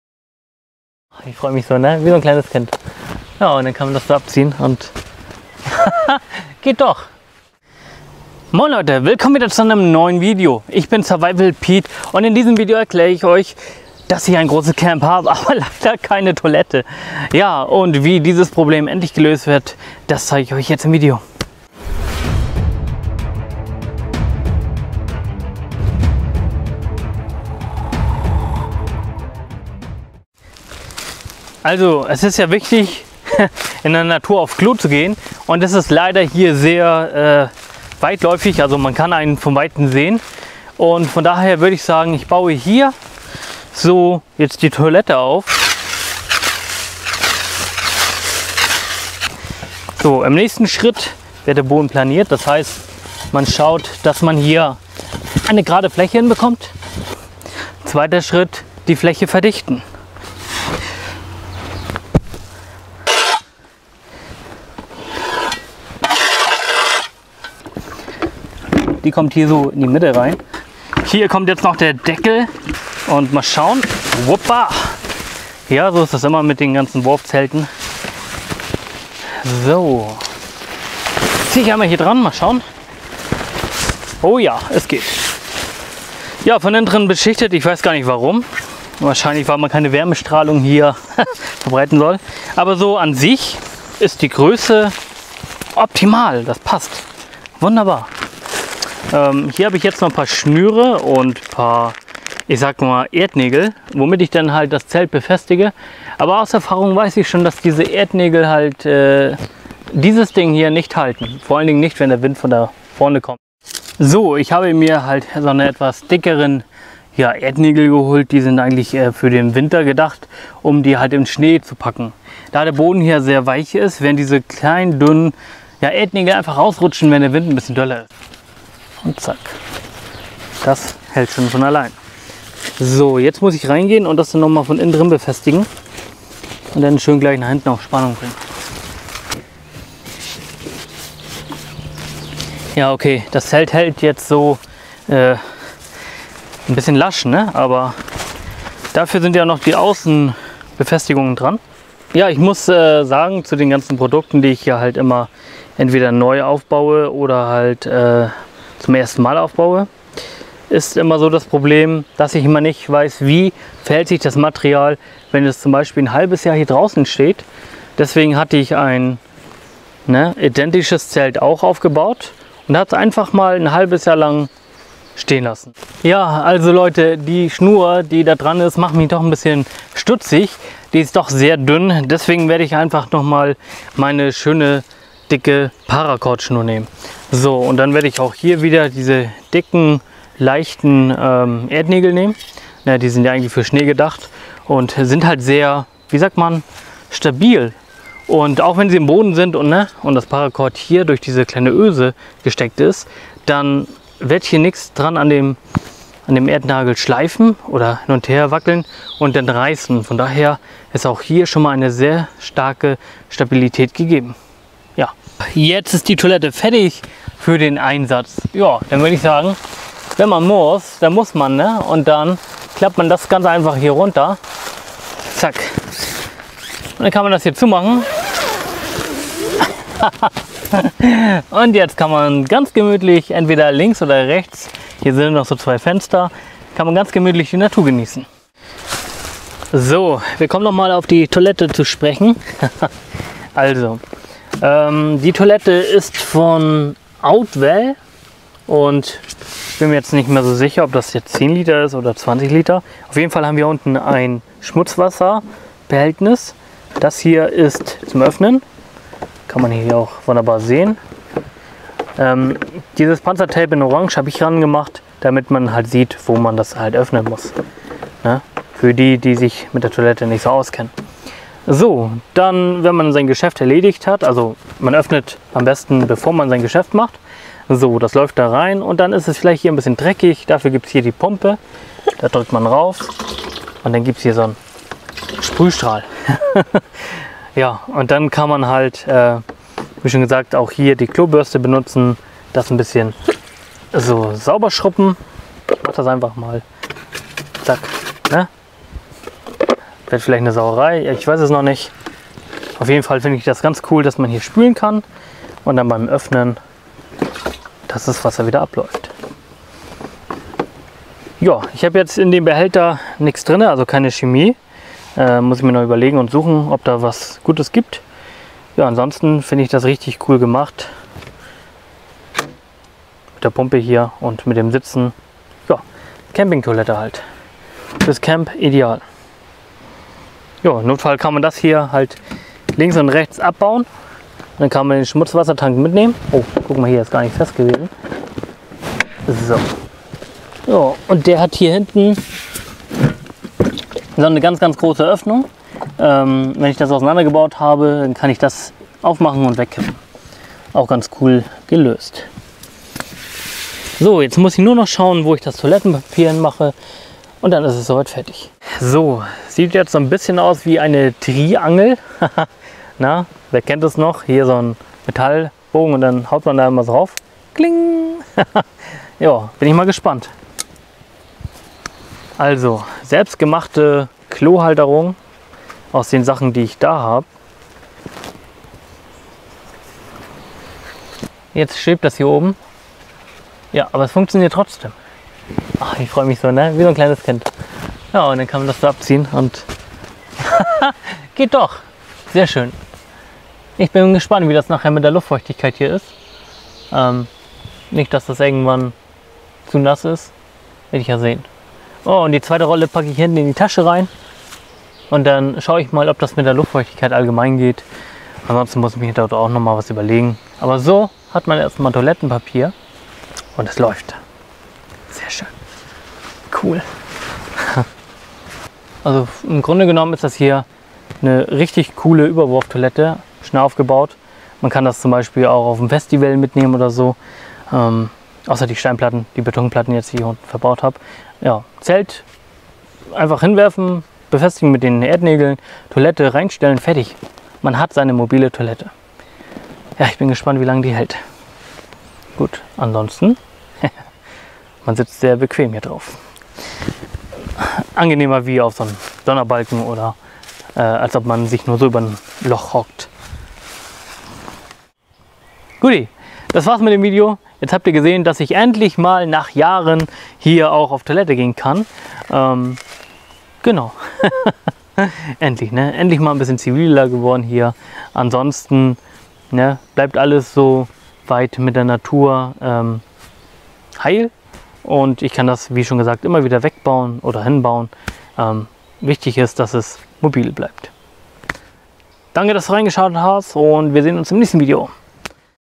Camping Toilette | Survival Piet
Seid ca. 3 Jahren habe ich keine vernünftige Toilette im Camp. Es musste schon das ein oder andere selbst gegrabene Loch hinhalten. Im Winter hatte man zwar freie Sicht, aber somit war man auch schnell enttarnt/entdeckt.

In diesem Video baue ich eine Mobile, als auch Stationäre Toilette auf.

Was ich genau verwende steht hier.

- 4x Gehwegplatte 50x50
- Toilette 20l
- Toilettenpapier Bio

In dem Abwassertank kommt 100ml Bio Toilettenzusatz rein.

Dieses ist zu 100% Ökologisch und kann ganz einfach entsorgt werden.(Es ist selbst für Gruben Toiletten geeignet.)